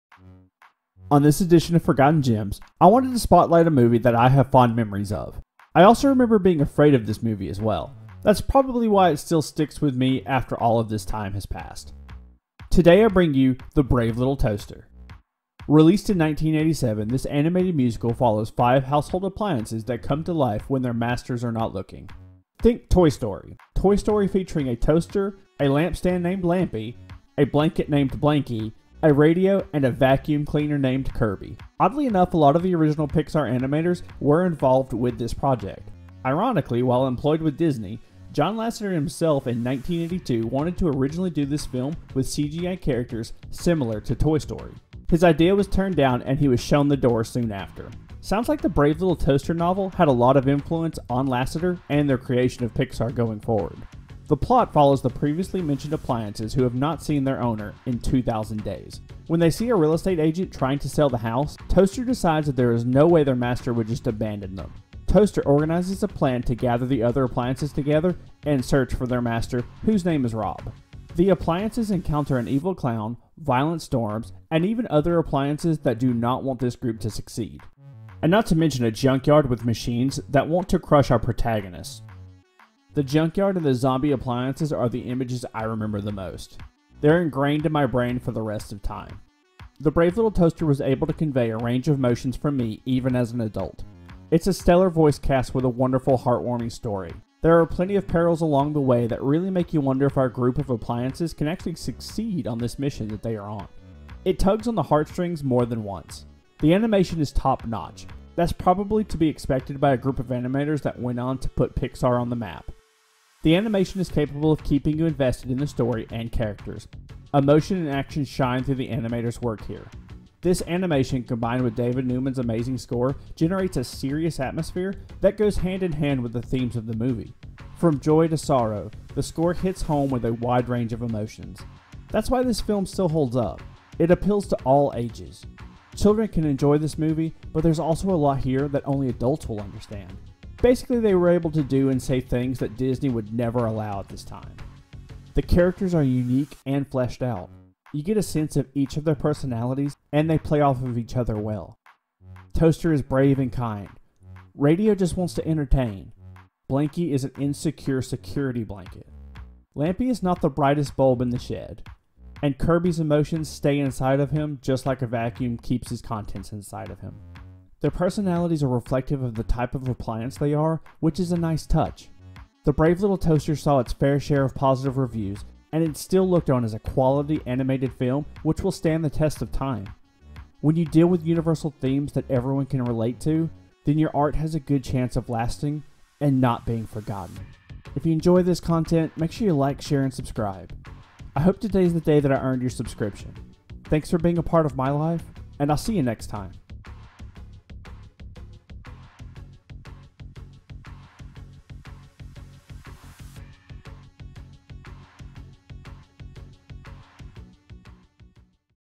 Forgotten Gems | The Brave Little Toaster | The move that inspired Pixar/Toy Story | Episode 6
In the sixth episode of my Forgotten Gems series, I am going over a film that still holds up today. It's still just as terrifying as I remember too.  Here is The Brave Little Toaster.